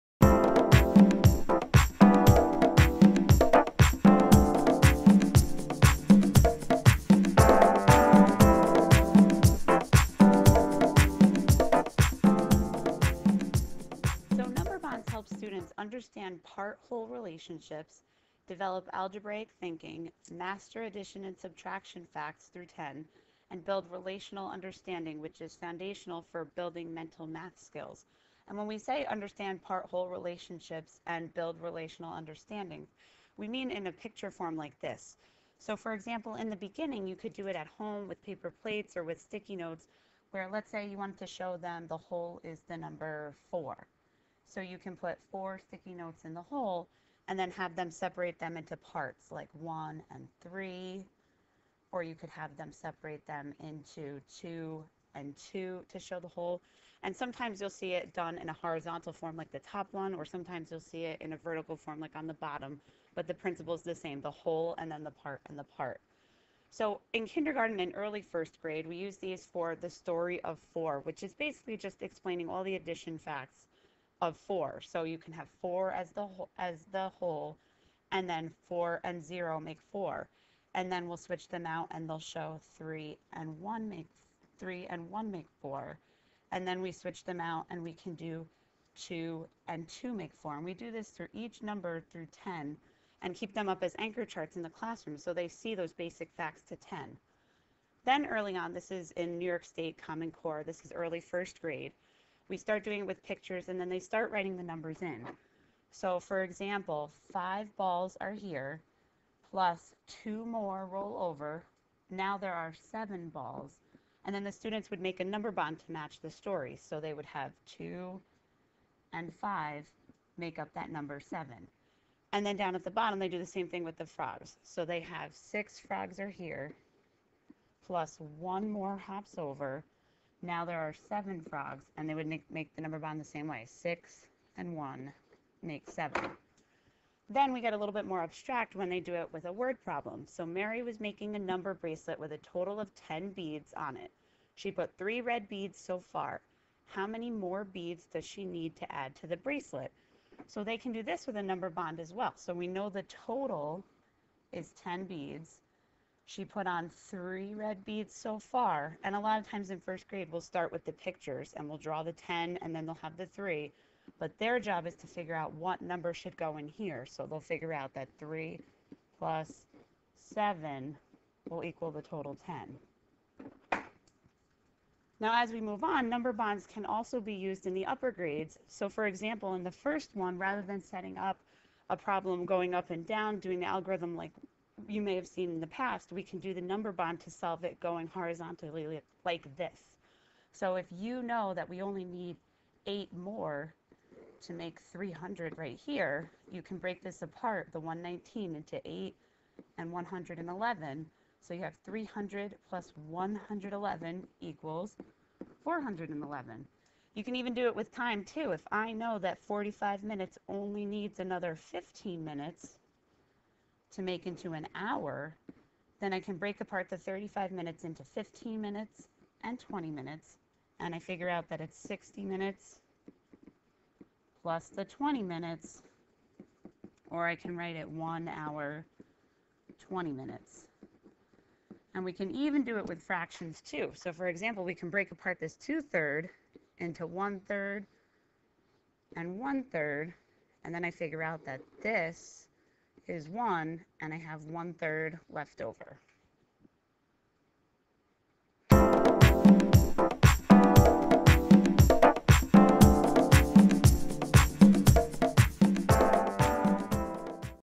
Number Bonds
See how we use number bonds in everyday Math lessons. Number bonds are pictorial representation of part-part-whole relationships. Number bonds consist of a minimum of 3 circles that are connected by lines. The "whole" is written in the first circle and its "parts" are written in the adjoining circles. Number bonds are used to build deeper understanding of math facts and help students understand part-whole relationships, develop algebraic thinking, master addition, subtraction, multiplication and division facts, and build relational understanding.